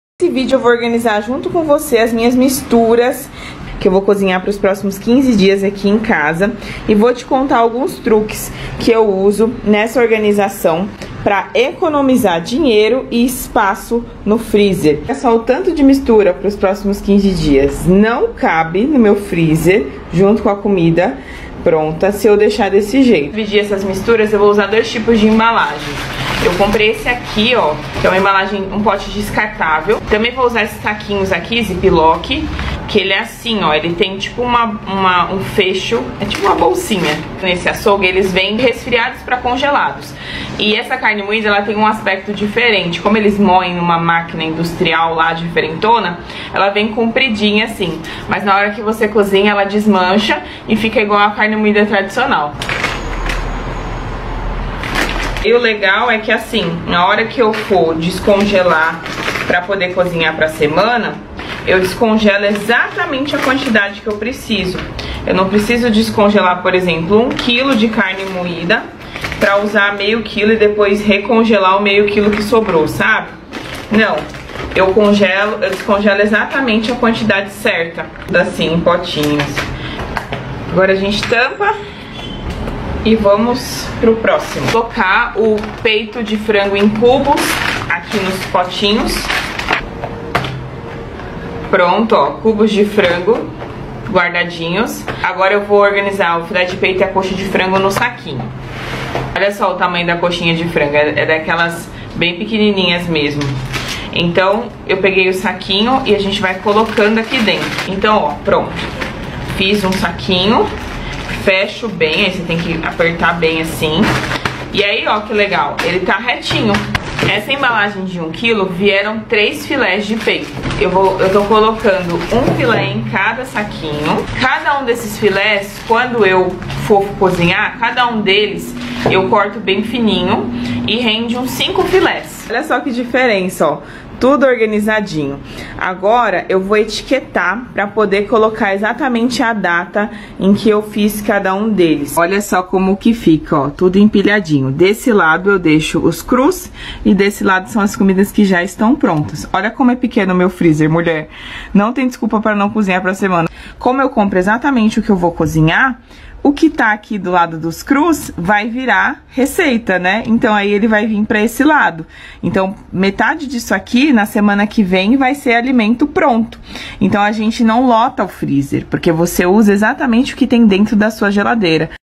Organizei as misturas do açougue pra 15 dias
Te ensino cozinhar pra semana toda e ter tempo livre pra você!